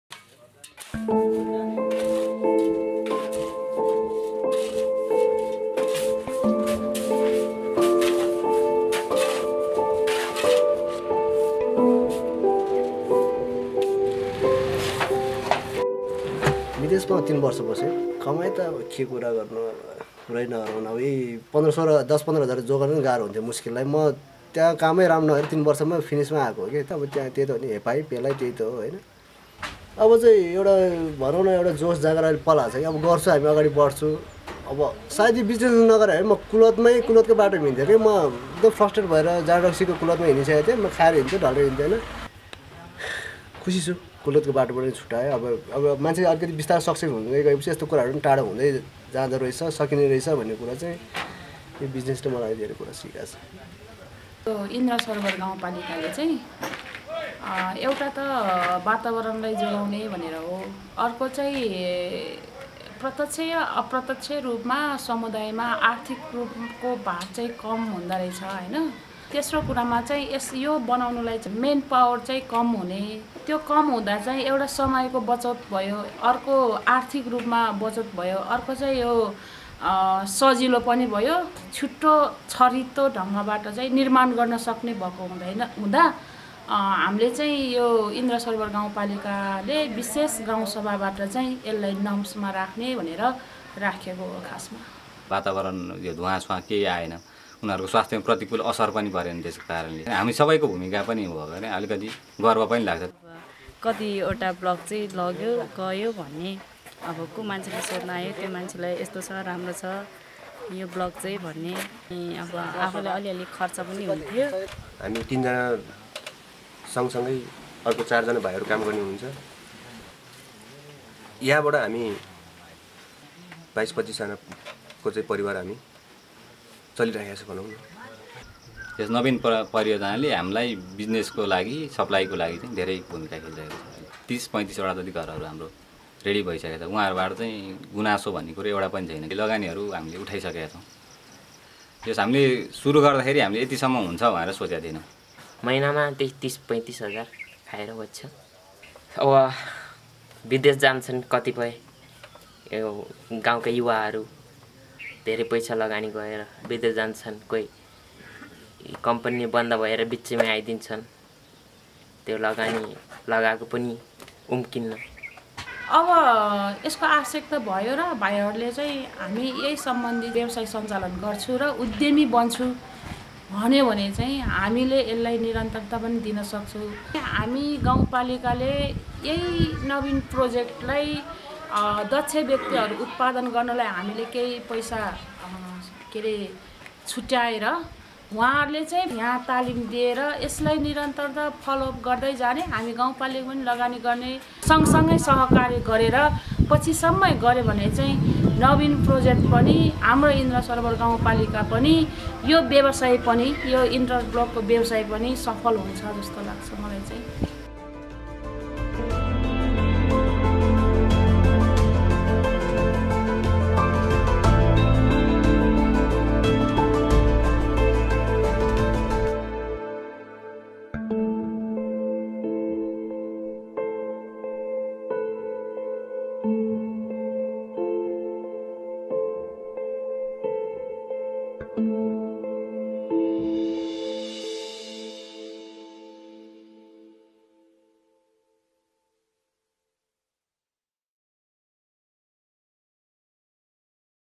NABIN Promoting Climate friendly CSEB enterprises
DCA, Community Impact Nepal, Build up Nepal and Practical action are working to support CSEB interlocking brick enterprises to start and produce climate friendly bricks.

Hear the story from some of the entrepreneurs and stakeholders.